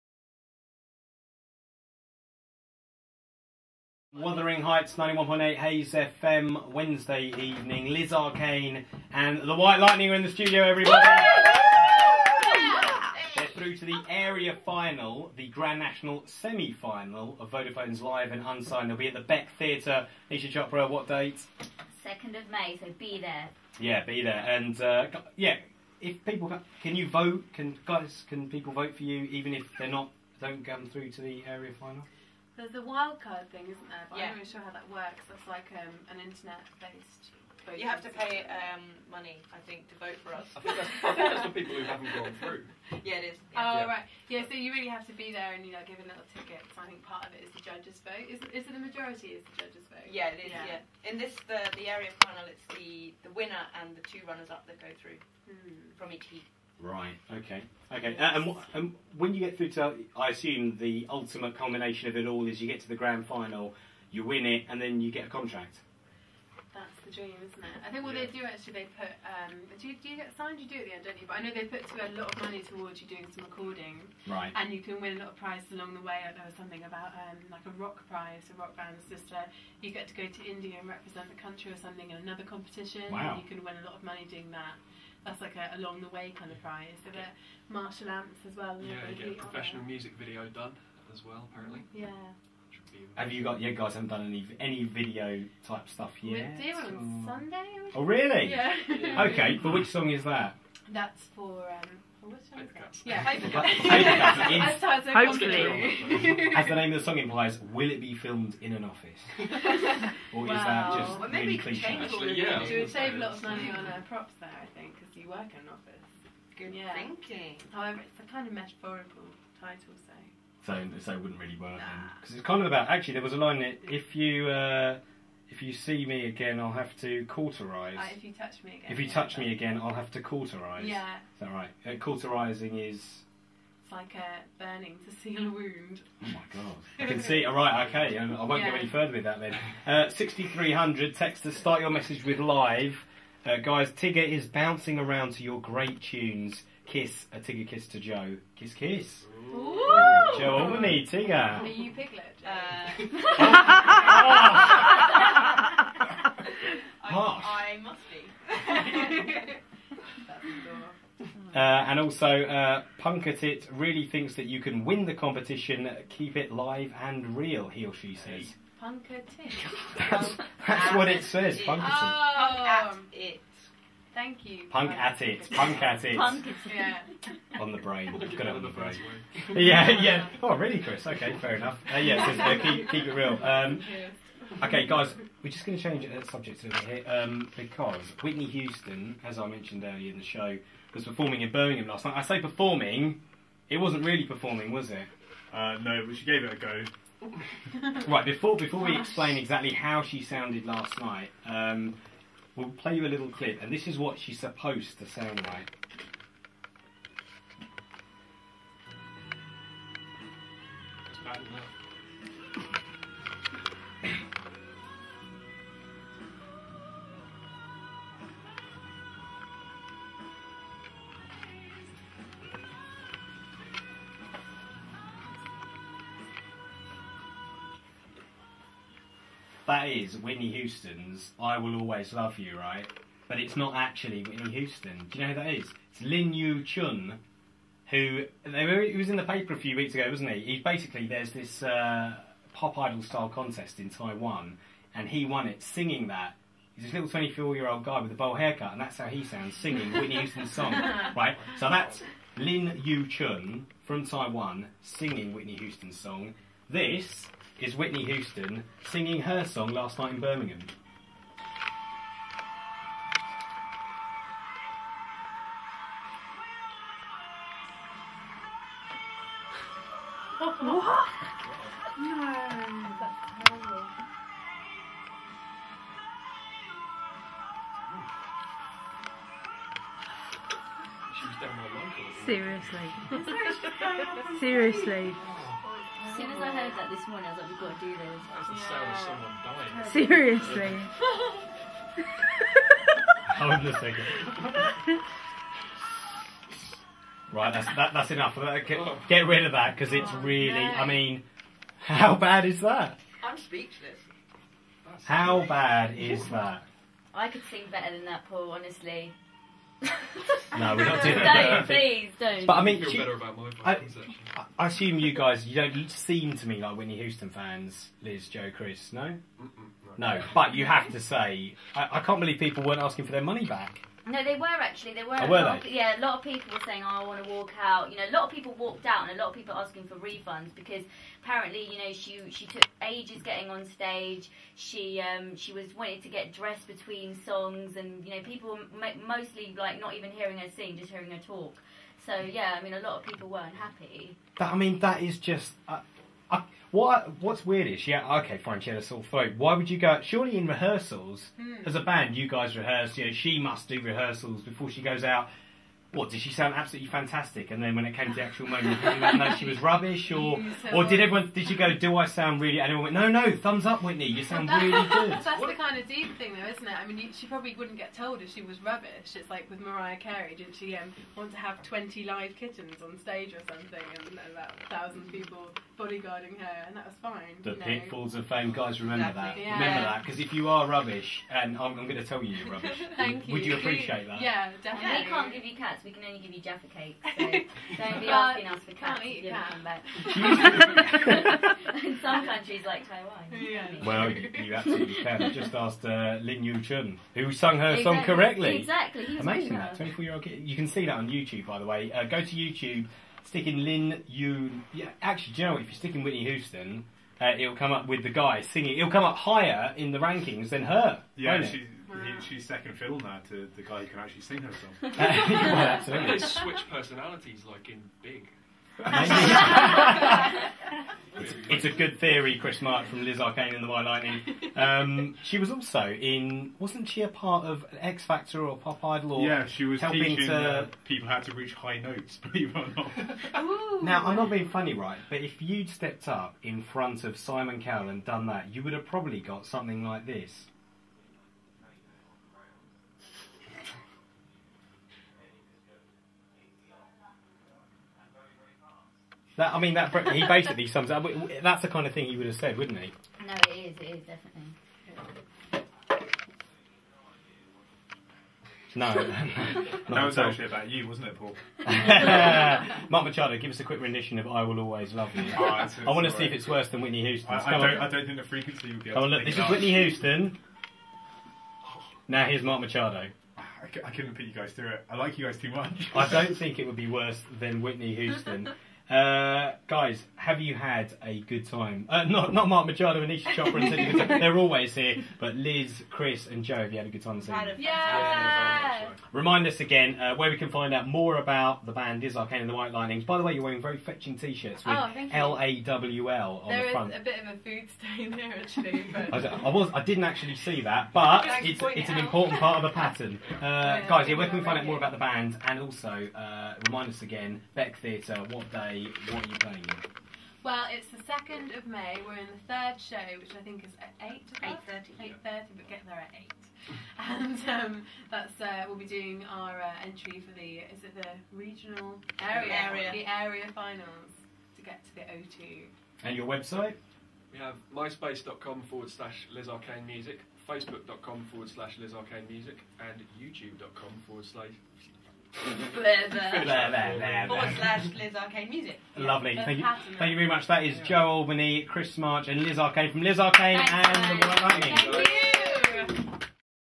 LAWL Hayes FM interview part 2
Liz Arcane and the White Lightning on Hayes FM interview part 2